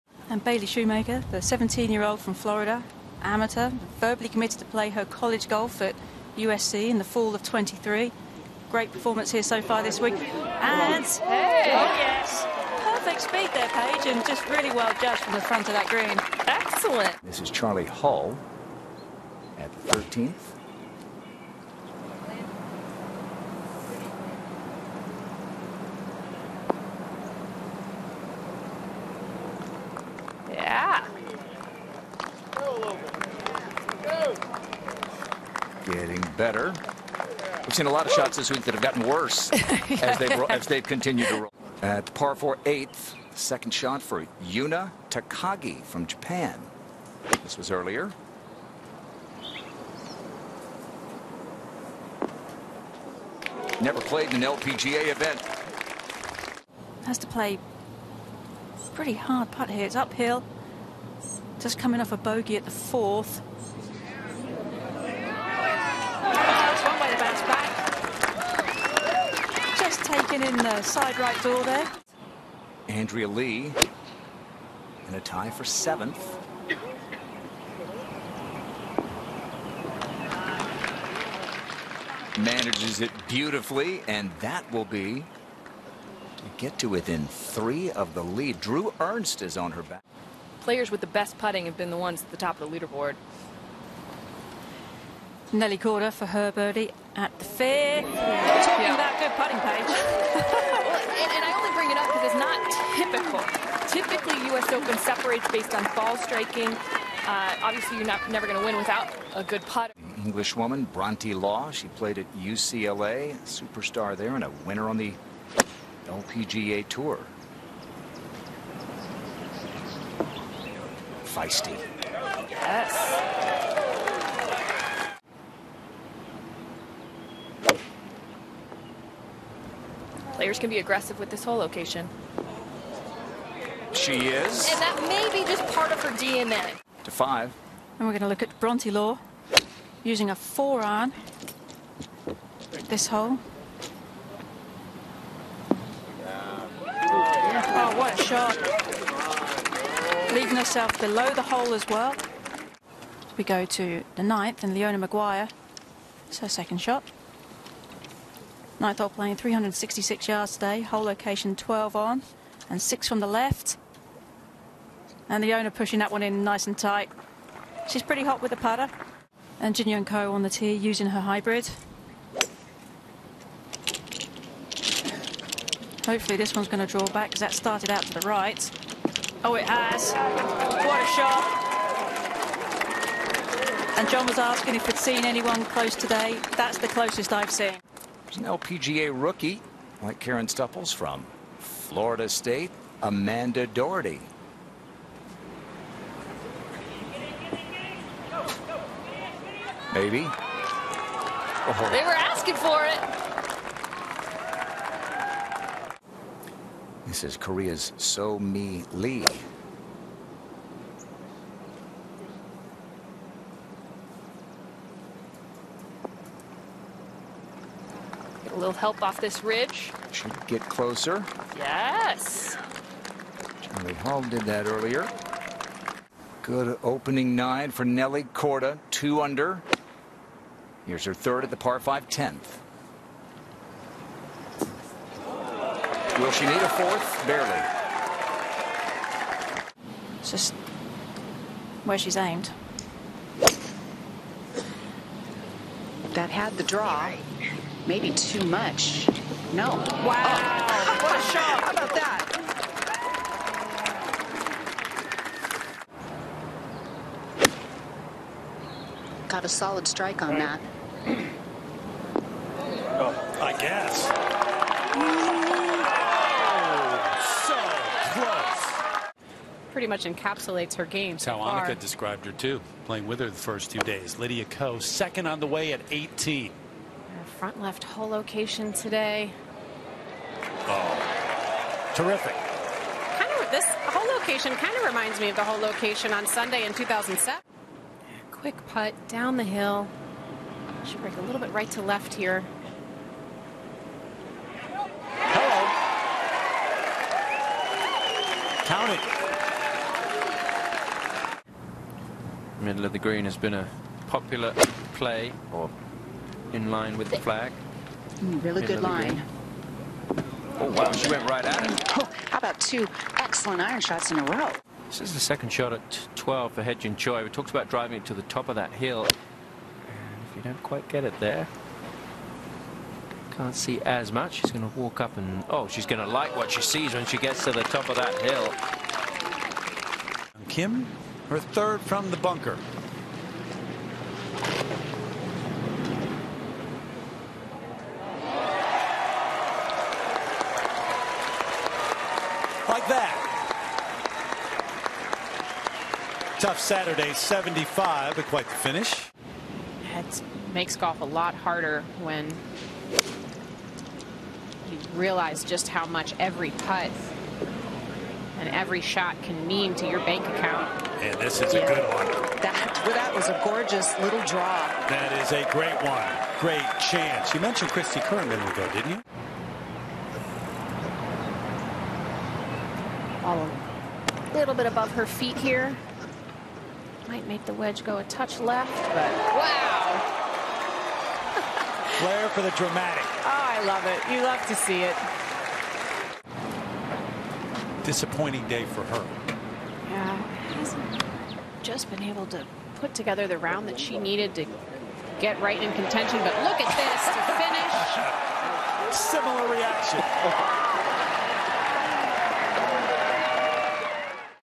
2022 U.S. Women's Open Highlights: Round 3, Top Shots
The world's best were on their games on Saturday at Pine Needles Lodge & Golf Club, in Southern Pines, N.C., during the third round of the 2022 U.S. Women's Open presented by ProMedica.